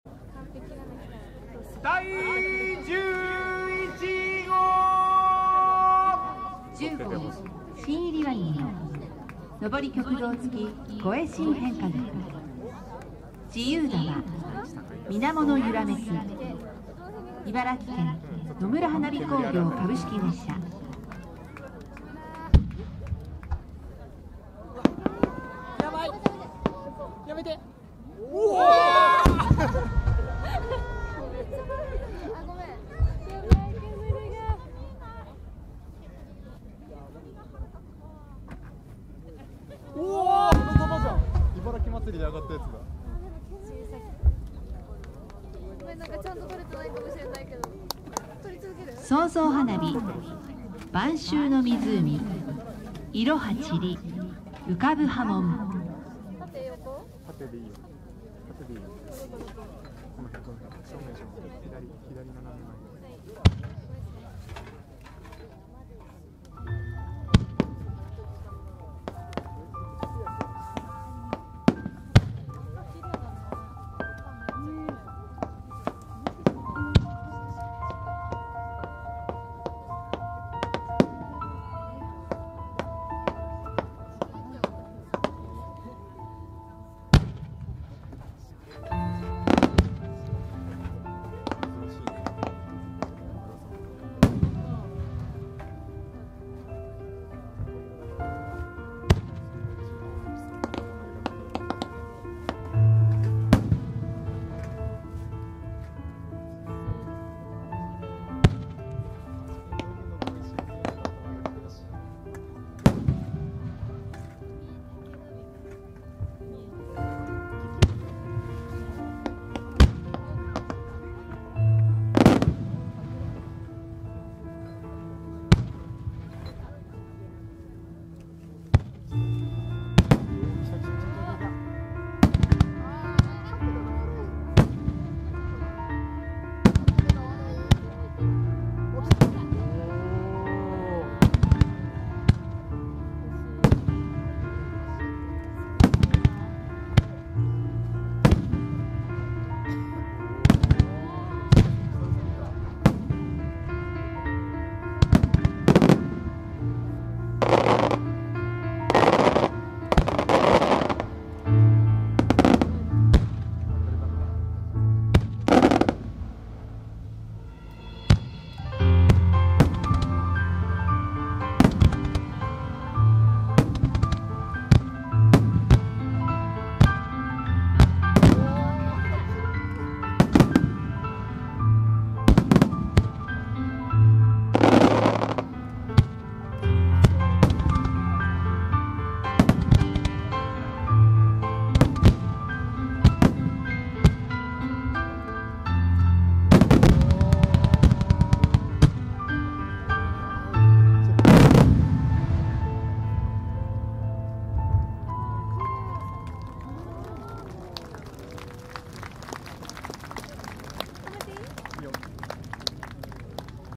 大曲の花火 2024 「No.11 野村花火工業」
・10号芯入割物「昇曲導付五重芯変化菊」
・10号自由玉　「水面のゆらめき」
・創造花火　　「晩秋の湖〜色葉散り、浮かぶ波紋〜」

【大会名】第96回全国花火競技大会(大曲の花火)
【開催場所】秋田県大仙市
【プログラム】No.11
【担当煙火】野村花火工業
【camera】DC-GH6